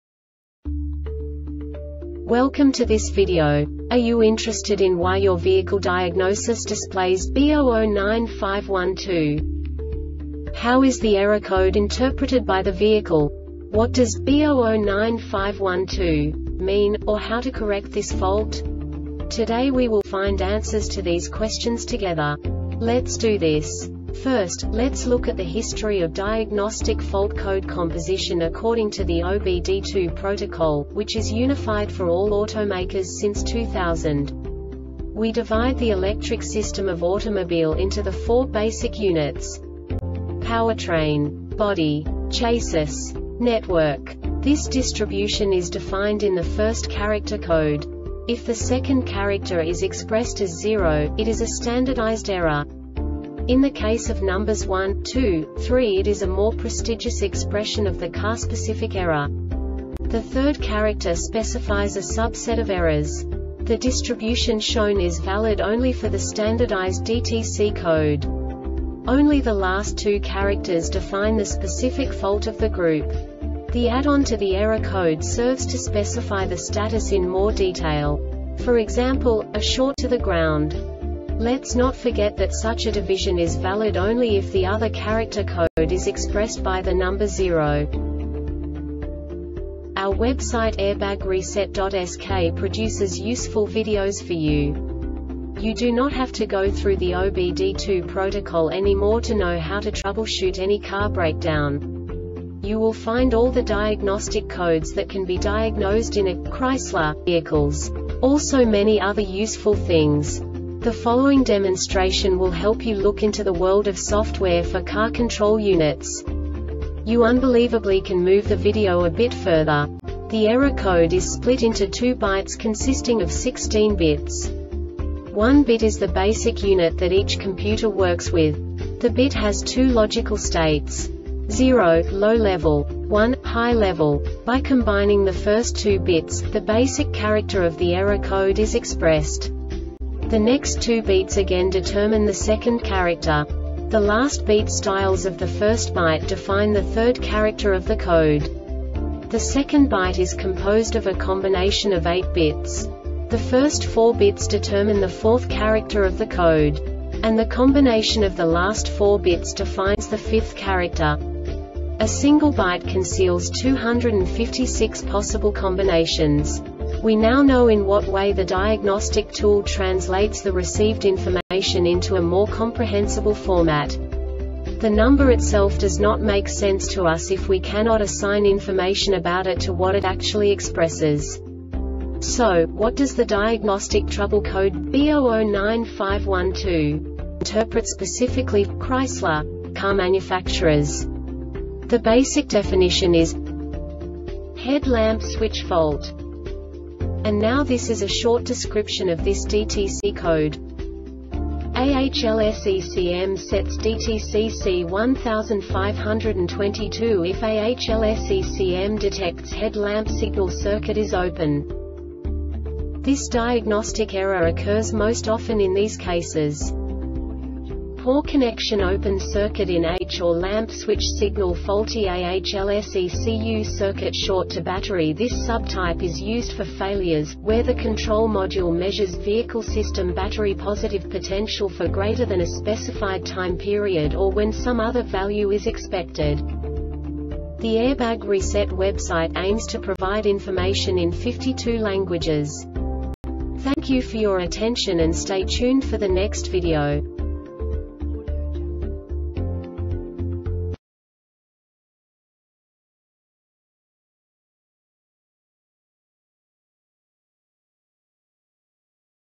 DTC Chrysler B0095-12 Short Explanation
Contents:
0:21 Basic DTC analysis according to OBD2 protocol standard.
1:48 Insight into programming
2:58 A diagnostically understandable description of the Diagnostic Trouble Code
3:05 Basic error definition

Make:
Chrysler
Code:
B0095 12
Definition:
Head lamp Switch Fault
Description:
AHLS ECM sets DTC C1522 if AHLS ECM detects HEAD Lamp signal circuit is open.
Cause:
 Poor connectionOpen circuit in H or Lamp switch signalFaulty AHLS ECU
Failure Type: